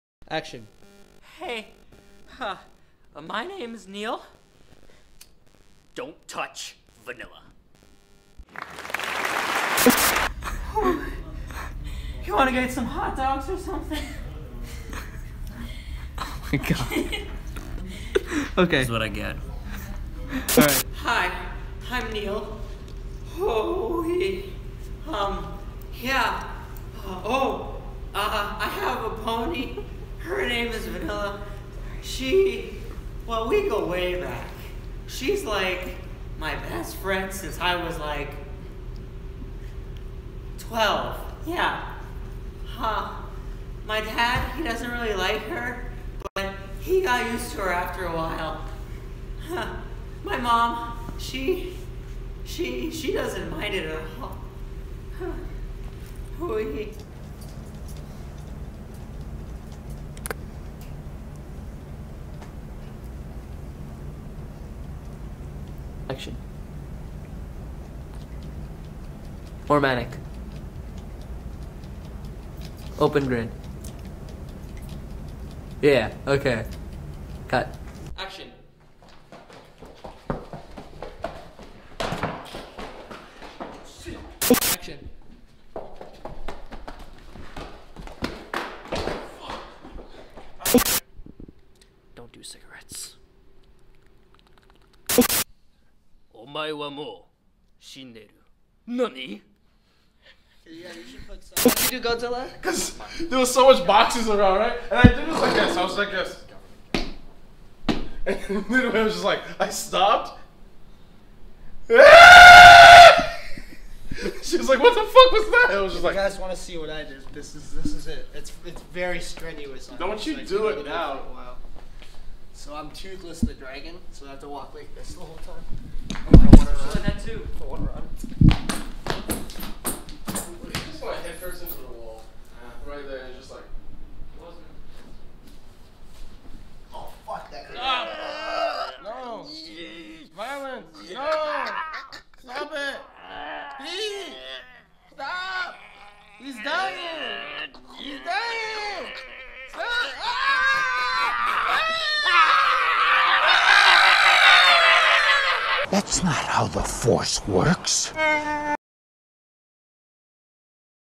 Term 2 Bloopers
Well, this is the blooper reel for all the videos I did in term 2 at college. There's a lot more of Neil Daffodil, my favourite original character that will probably never actually get a publicly released film... Special thanks to Luca Garib, the guy who really brought this character to life like no one else could.